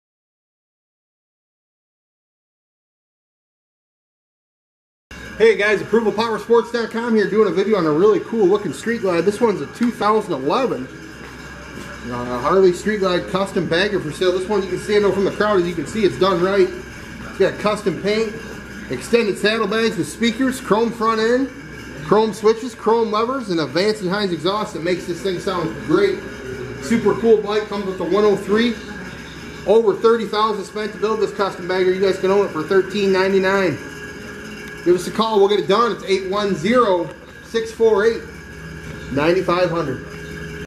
2011 HARLEY STREET GLIDE CUSTOM U5490
USED HARLEY STREET GLIDE CUSTOM BAGGER FOR SALE! STAND OUT FROM THE CROUD!! THIS ONE IS DONE RIGHT, EXTENDED SADDLEBAGS WITH SPEAKERS, CHROME FRONT END, CHROME SWITCHES, CHROME LEVERS AND THE VANE G& HINES EXHAUST SOUND GREAT! TURN HEADS EVERYWHERE YOU GO! OVER $30,000 SPENT TO CUSTOMIZE THIS BAGGER!